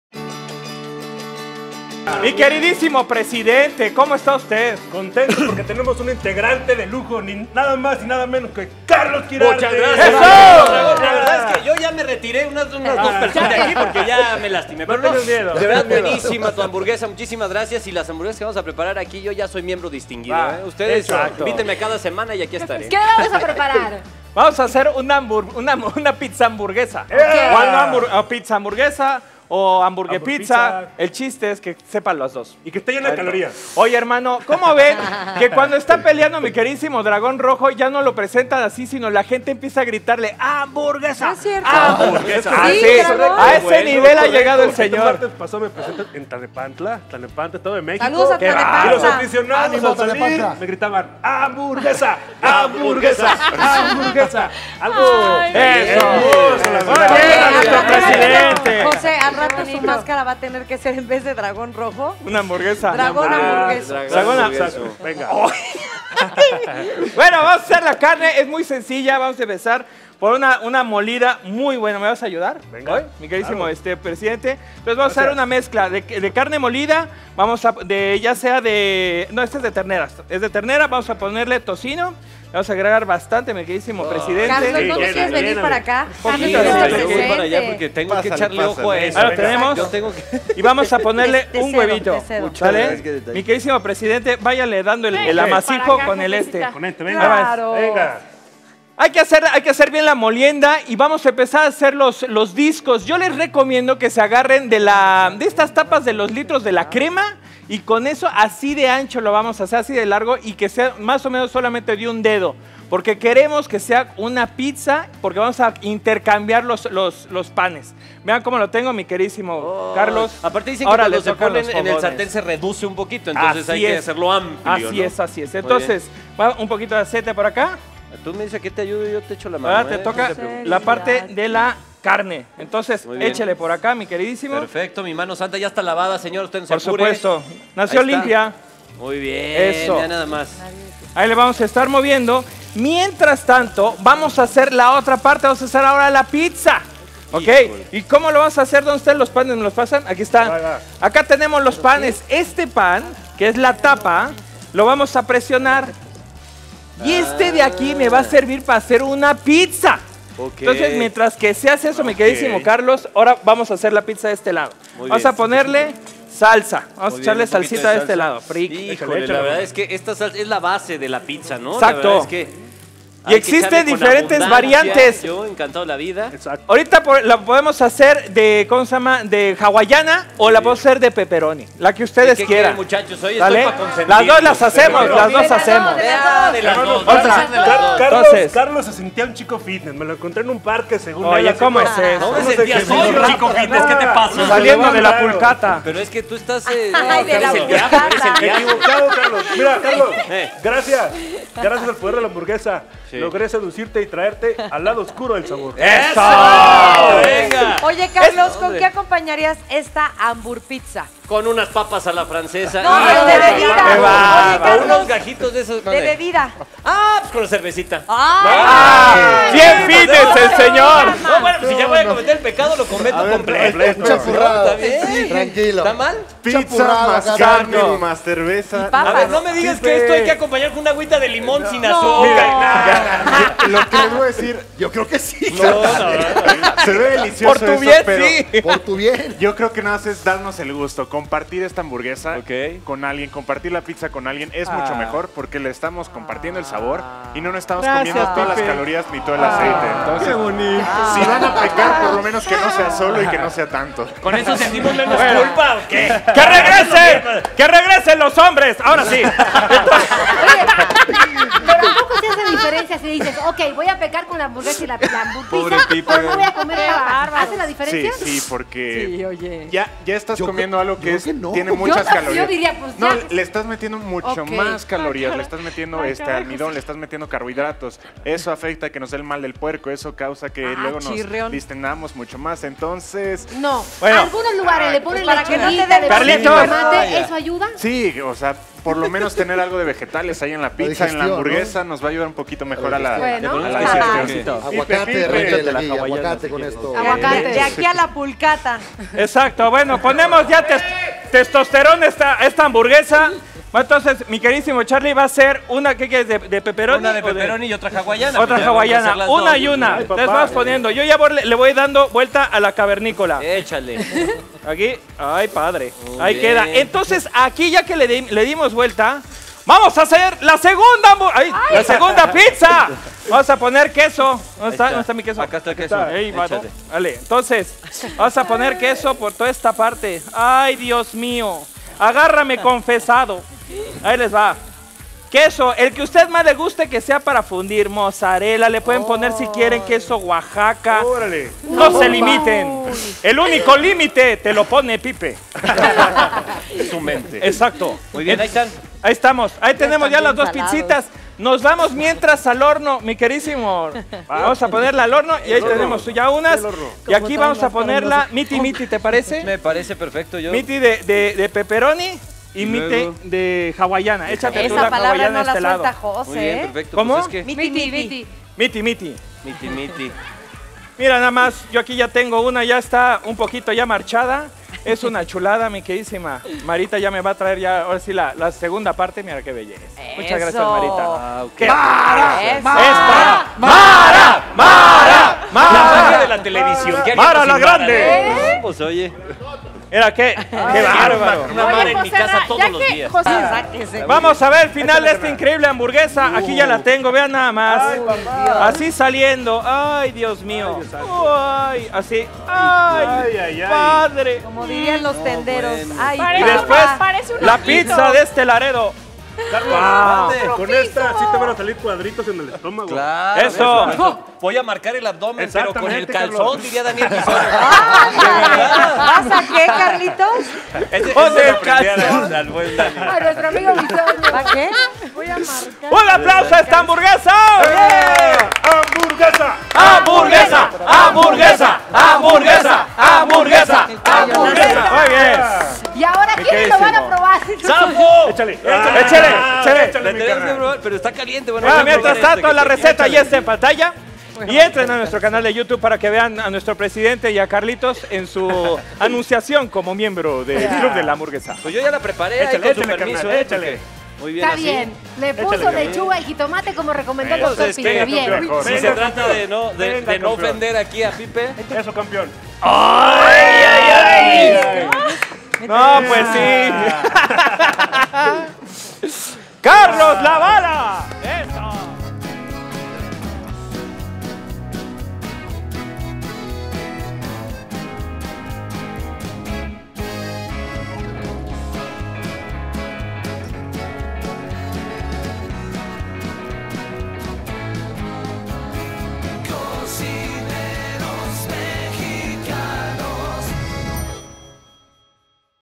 Receta: Hamburpizza | Cocineros Mexicanos
La combinación perfecta de dos de los mayores antojos, se unen para crear la Hambur-pizza... ¡Un pecado lleno de sabor!

1 Pan para hamburguesa
200 gr. de queso mozzarella
50 gr. de carne de cerdo molida
50 gr. de carne de res molida
40 gr. de tocino picado
1 huevo
60 gr. de jamón York
30 gr. de piña
50 gr. de salsa de tomate Italiana
MANERA DE HACERSE
​1.​Mezcla las carnes con el tocino y huevo, moldea la carne de forma que quede anchas y no muy gruesa, sella por ambos lados.
​2.​Coloca la carne sobre el pan, la carne, un poco de salsa de jitomate, queso, jamón y piña, tapa con el otro pan, coloca salsa de jitomate y queso, gratina.
​3.​Acompaña la hamburguesa con lechuga y jitomate en la parte de abajo de la carne.